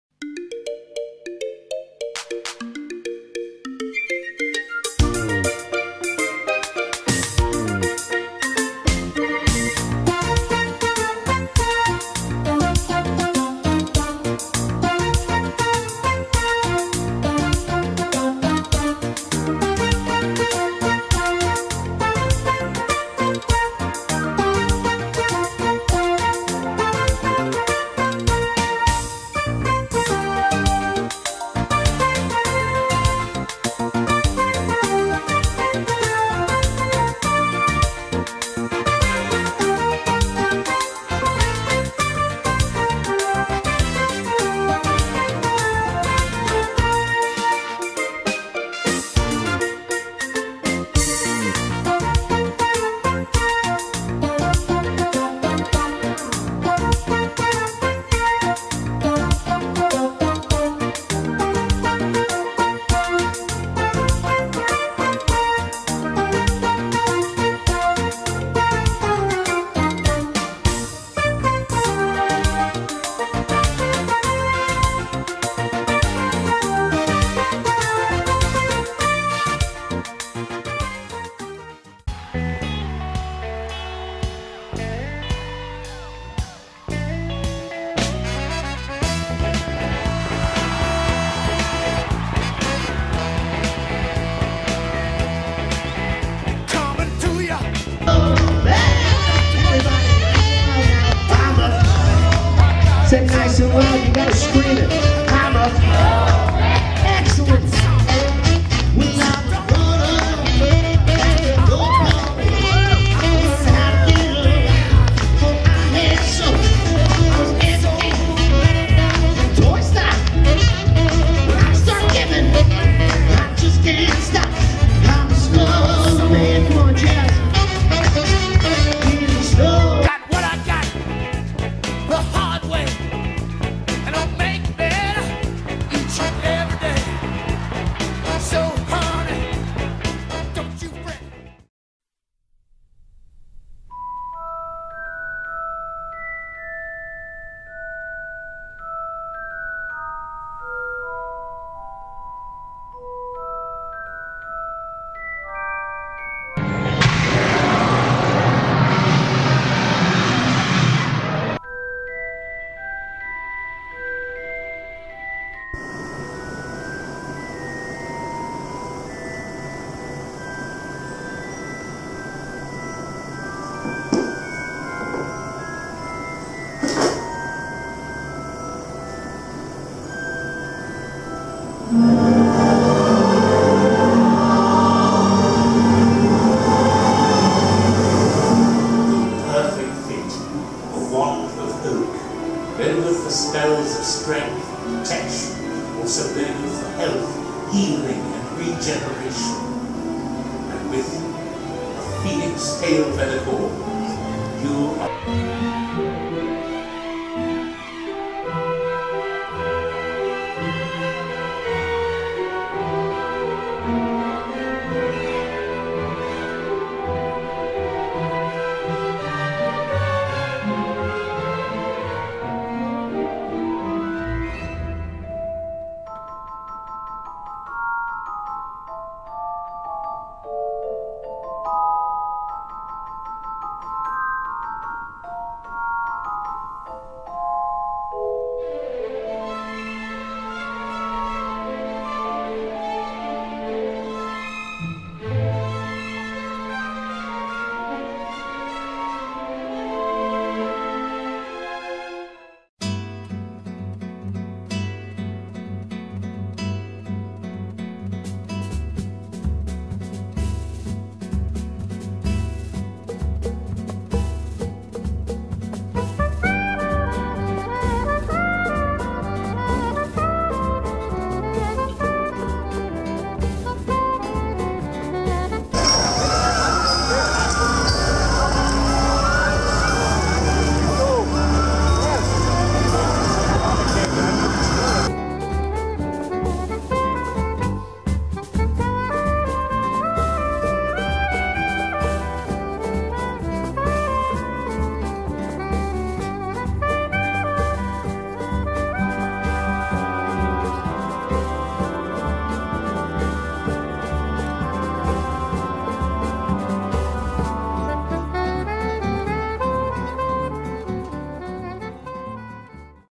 Georgia Aquarium December 2017
We spend the day at a excellent aquarium in downtown Georgia.